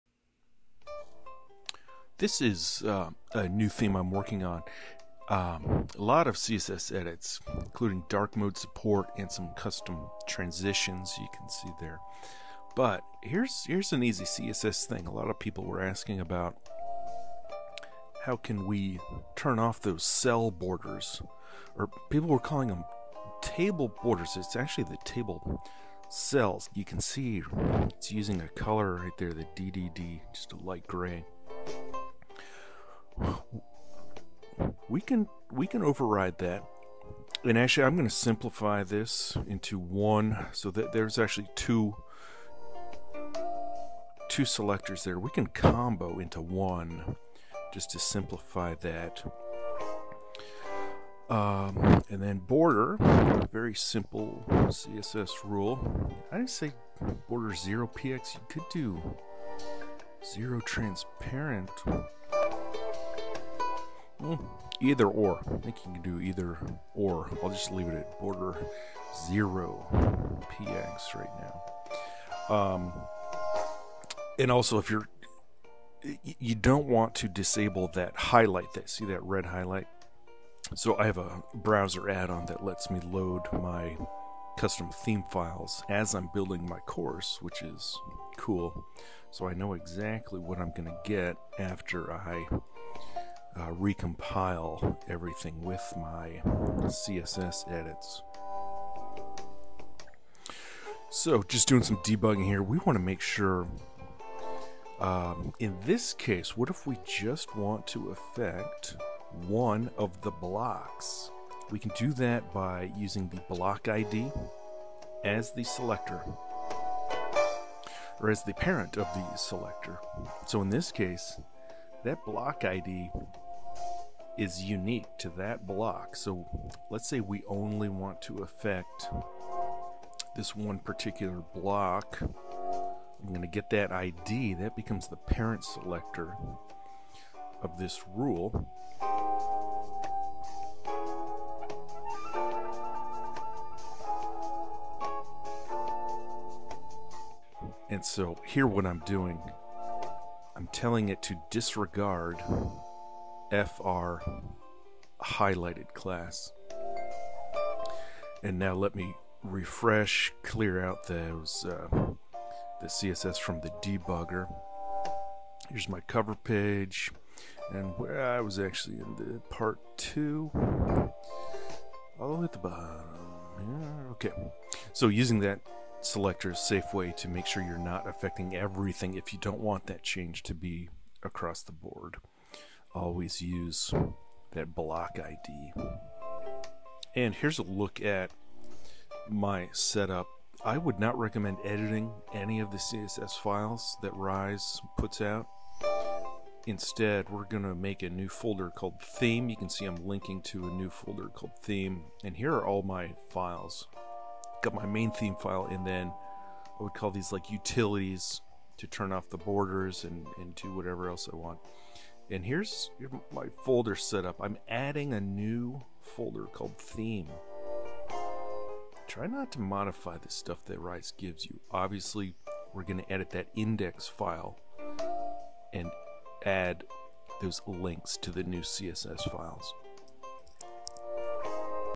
Articulate Rise 360- customize your theme with CSS
Clearing out the table cell borders, and using block ids to localize your css edits.